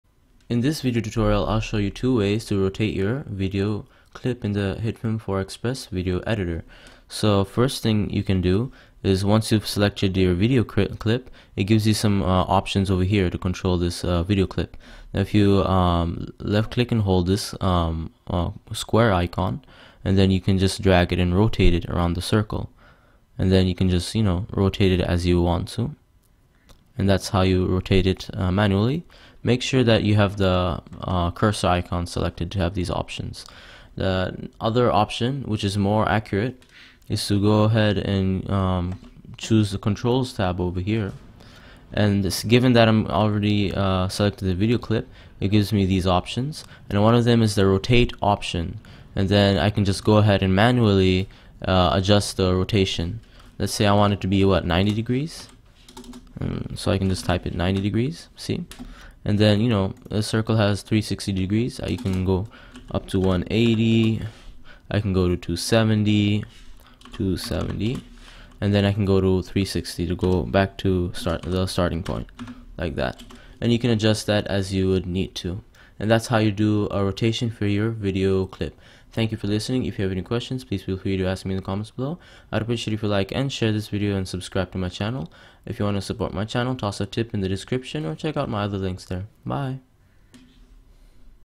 How to Rotate the Angle of your Video Clip in Hitfilm 4 Express
In this video tutorial, I will show you how to rotate your video clip in the hitfilm 4 express video editor. This is useful if you want to fix a video that you have taken upside down, or at some weird angle. This may also come in handy for any creative specific effect that you want apply to your video while you edit it.
Video Shooting Guide ➝
(づ⌐■ ͜ʖ■)づ Cool Amazon Gadgets